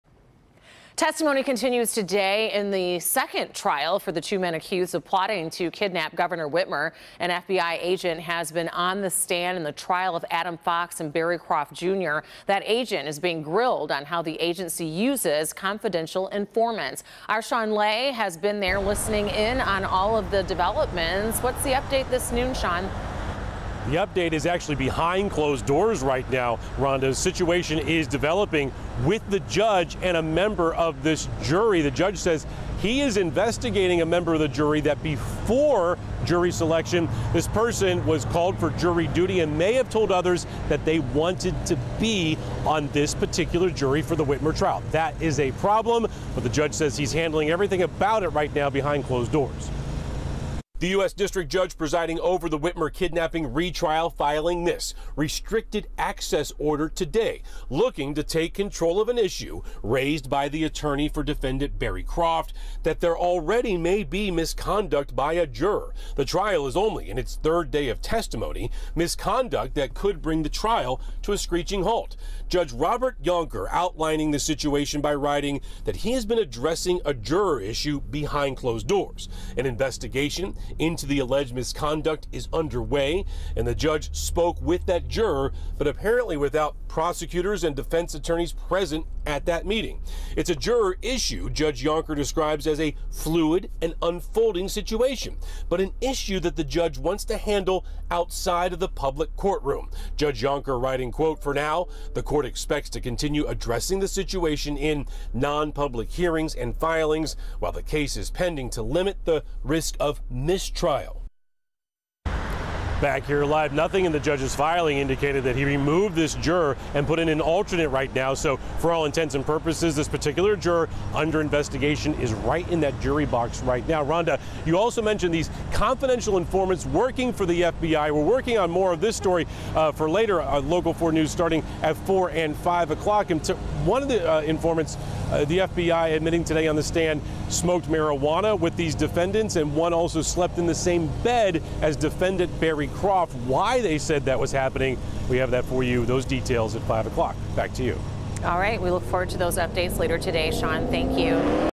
Lawyer in Whitmer kidnap trial raises concern about juror
U.S. District Judge Robert Jonker said any subsequent court filings about the issue will be sealed from the public and attorneys can't talk publicly about it. READ